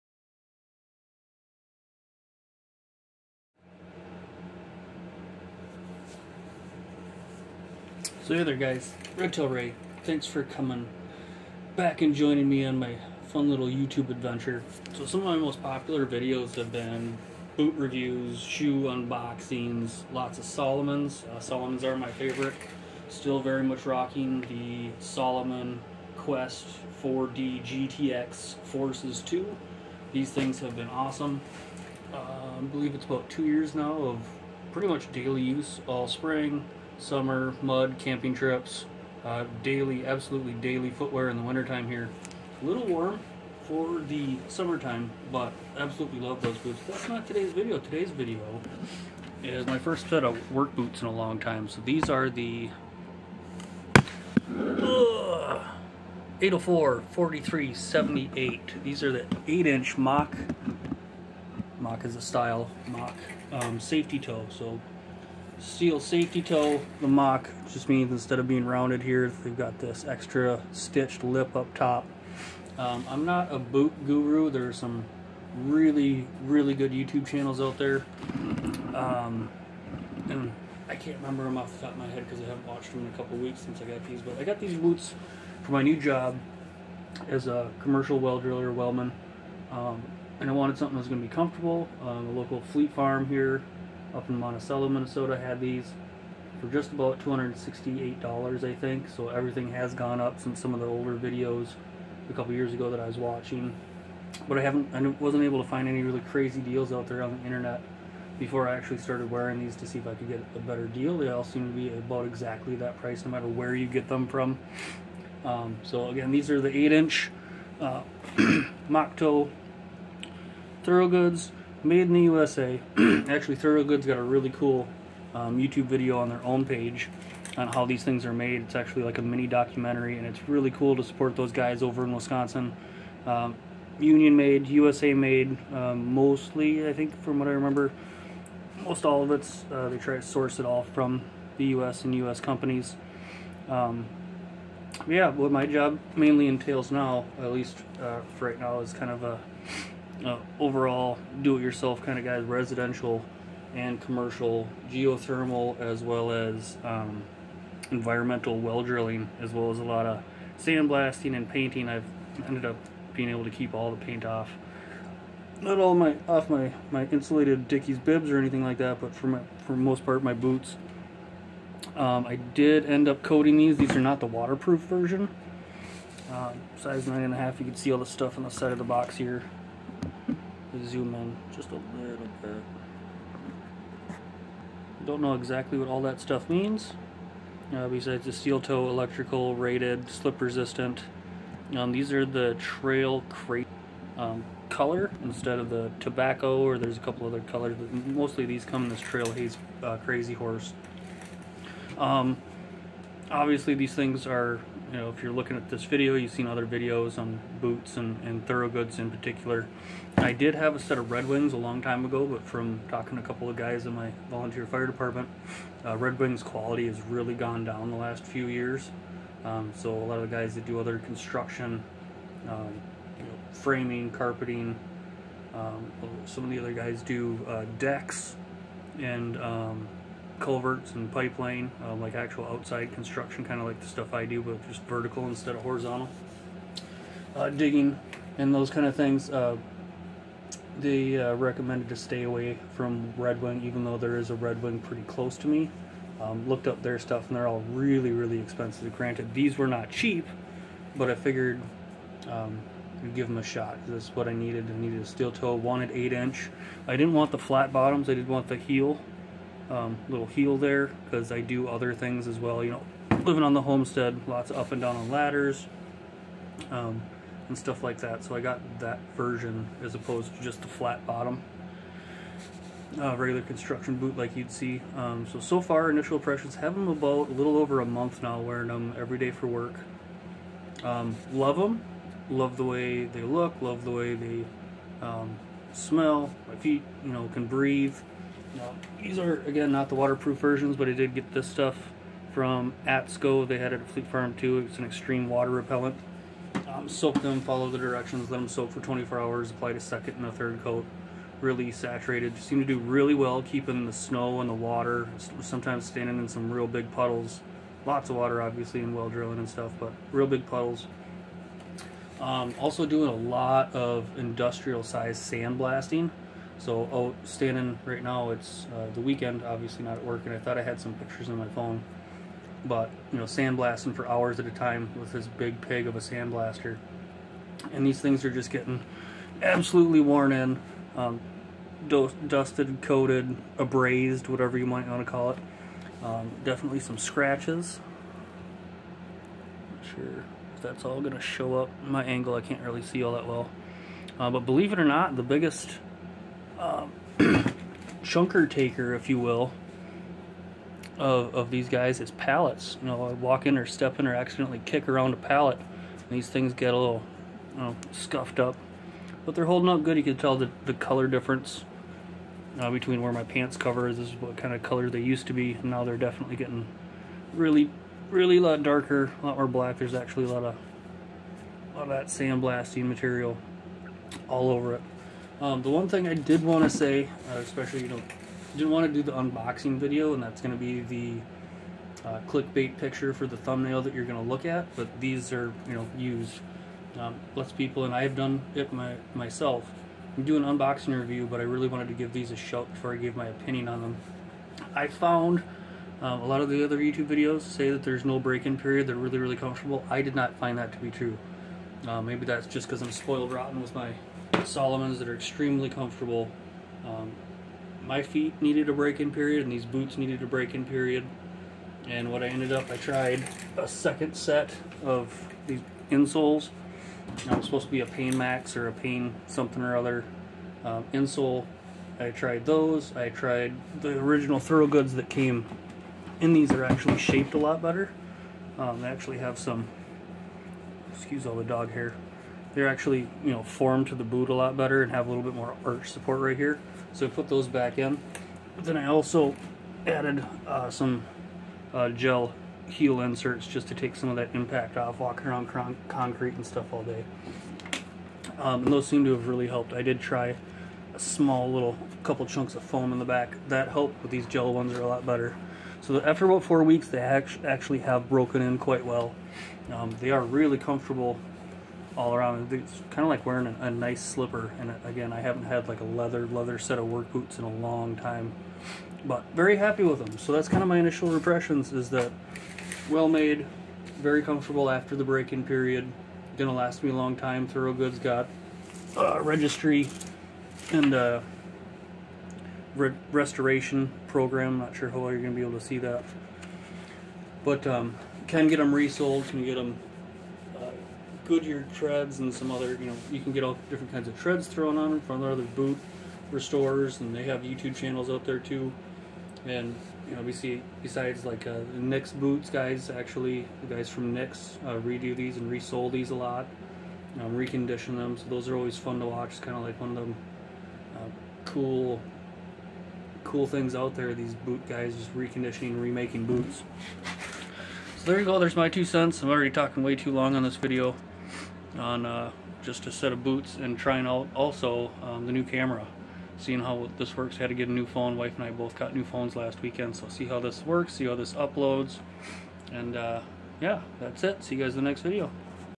THOROGOOD 8” MOC SAFETY TOE REVIEW
1 month review of my new work boots.